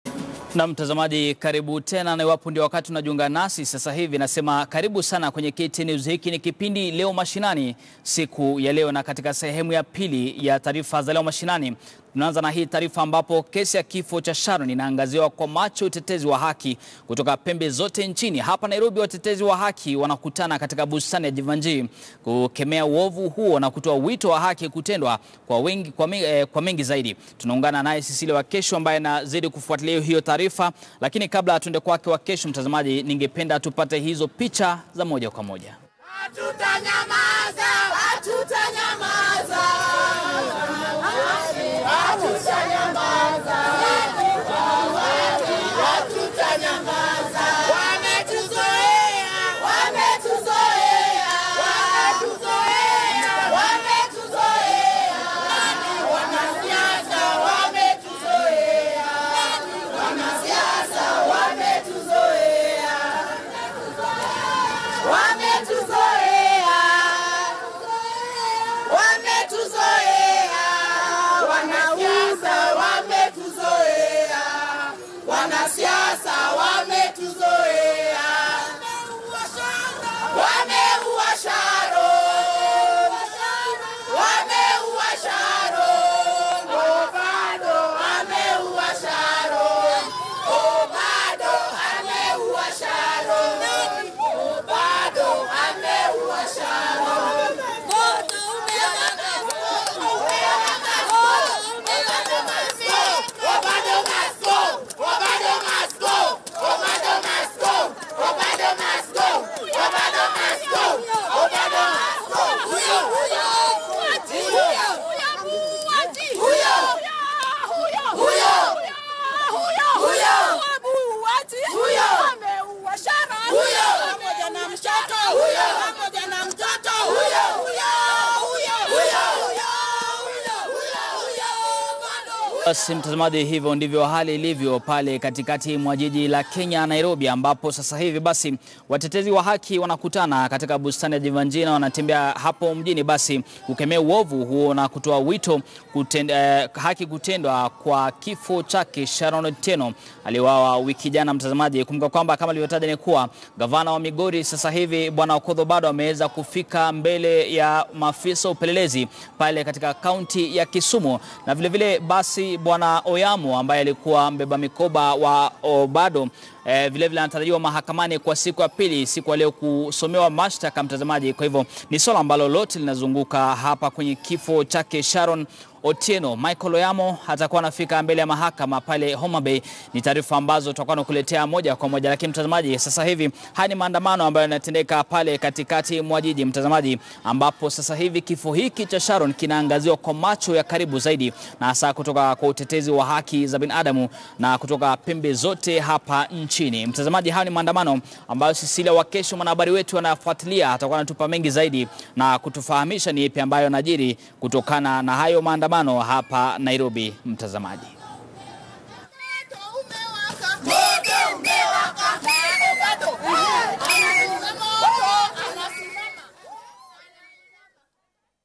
Wabunge wa kike waandamana kukemea vikali mauaji ya sharon otieno
Kesi ya kifo cha sharon yaangaziwa kwa macho na watetezi wa haki.